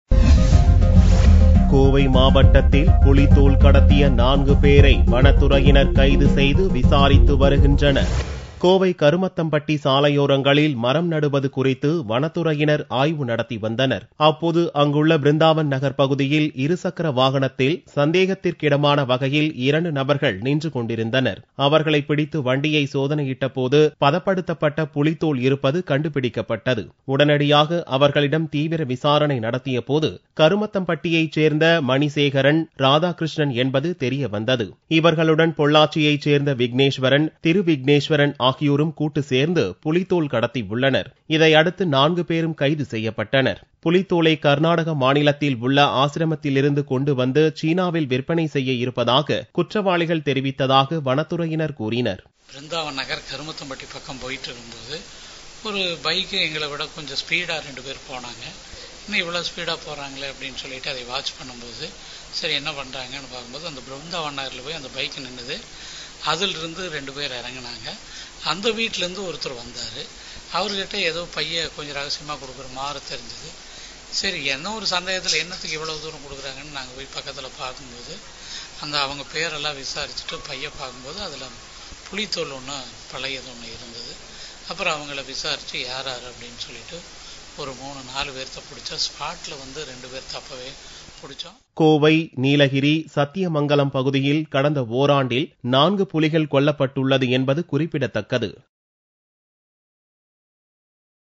4 People Arrested in Kovai for having Tiger skin with them - Dinamalar June 15th News
4 People Arrested in Kovai for having Tiger skin with them - Dinamalar June 15th 2014 Tamil Video News  For more videos